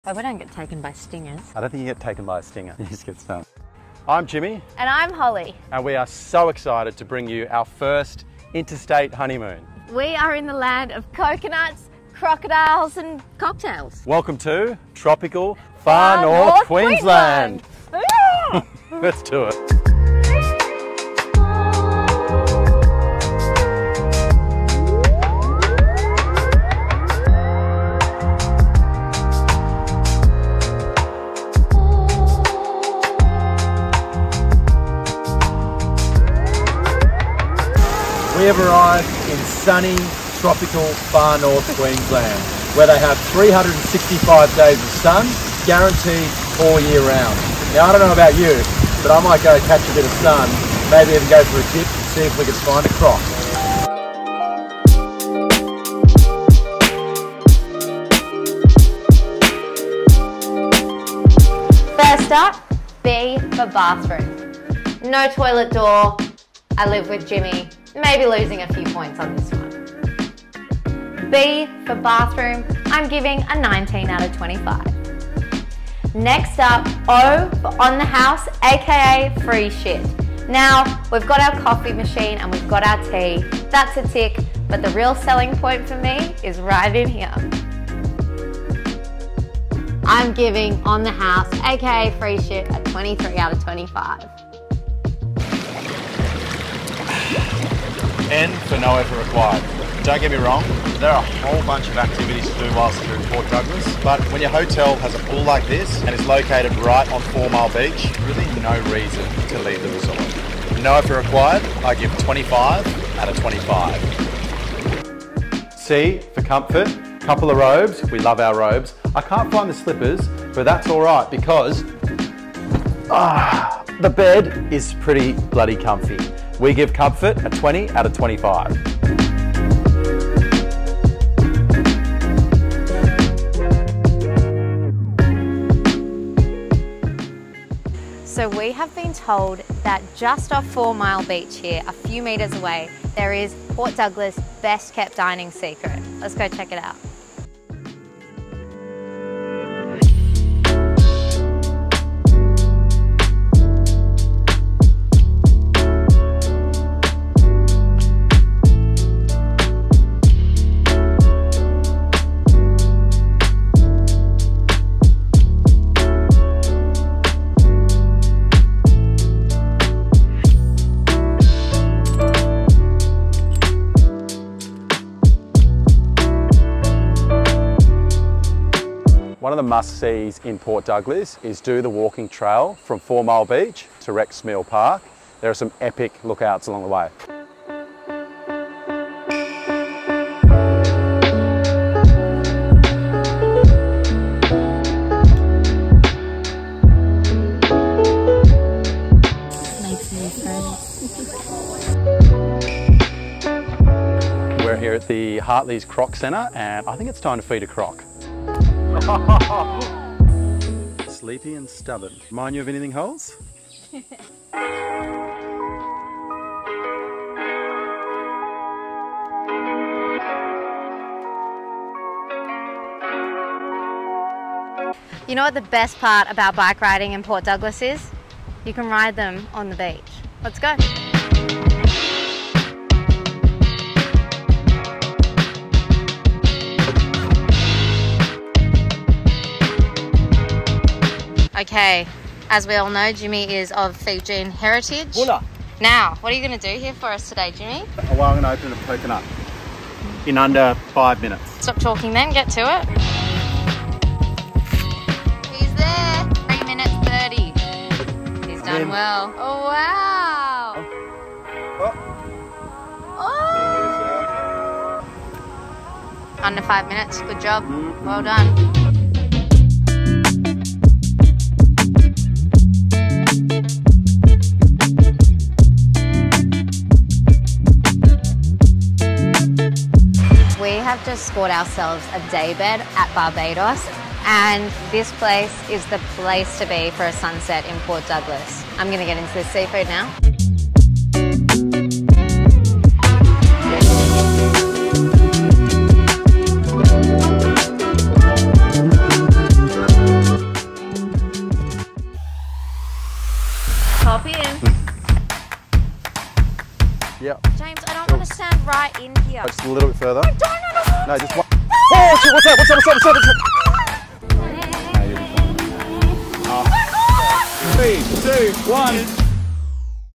100 Honeymoons Episode 3 - Port Douglas
Jimmy and Holly head off on their first inter state honeymoon. From feeding crocs to feeding Jimmy, this episode will show you the best of Port Douglas.